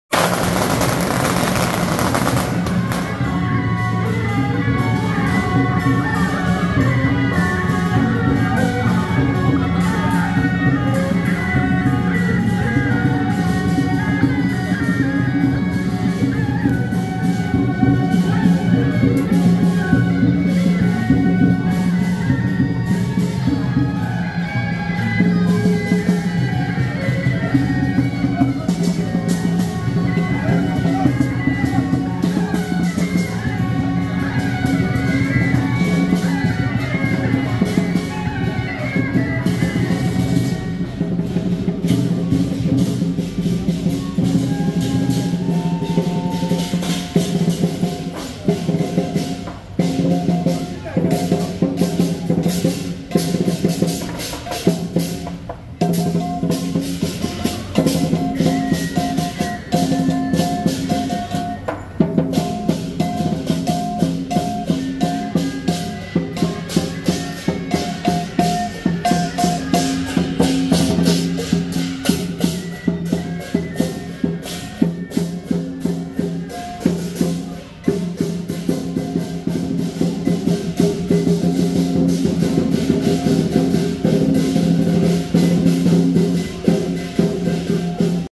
【台北慶和舘-醒獅團】~112艋舺啟天宮恭祝天上聖母聖誕遶境
台北慶和舘-醒獅團